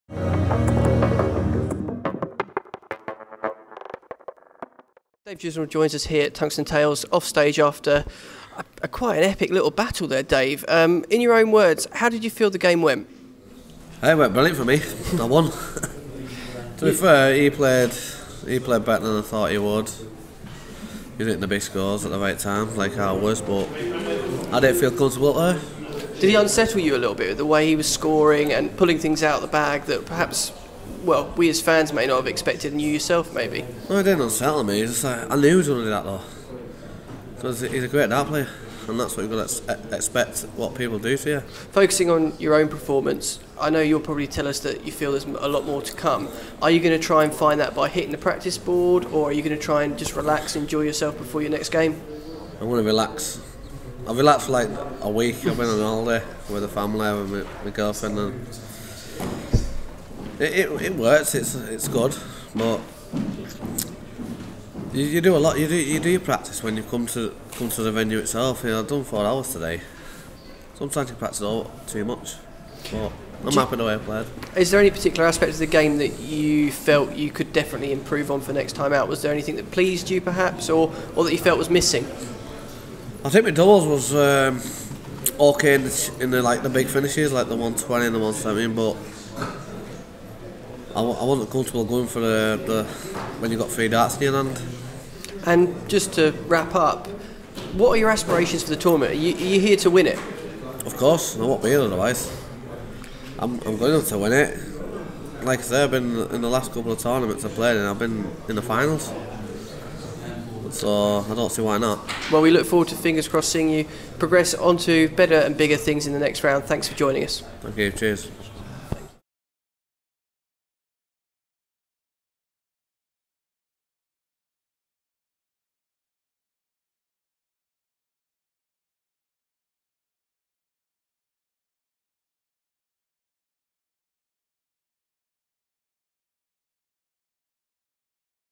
Dave Chisnall v Rowby John Rodriguez | "He played better than I thought" | 3-2 Win
Dave Chisnall fresh after a 3-2 against Rowby John Rodriguez at The William Hill World Darts Championship 2017